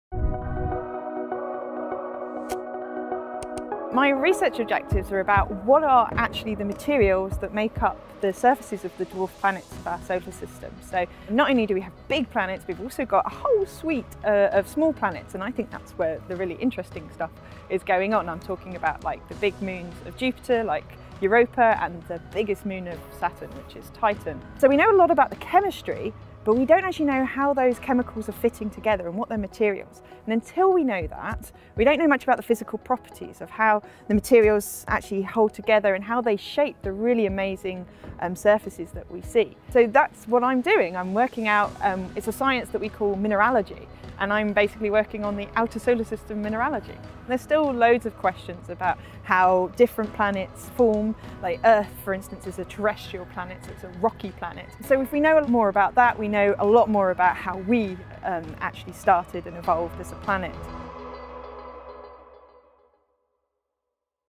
Meet the Scientist - Dr Helen Maynard-Casely
Dr Helen Maynard-Casely is an instrument scientist working on the WOMBAT high-intensity powder diffractometer at ANSTO’s Australian Centre for Neutron Scattering. Helen’s research is seeking to better understand the makeup of the surface materials of the dwarf planets of our solar system including the surface materials of Europa, a large moon of Jupiter, and Titan the largest moon orbiting Saturn.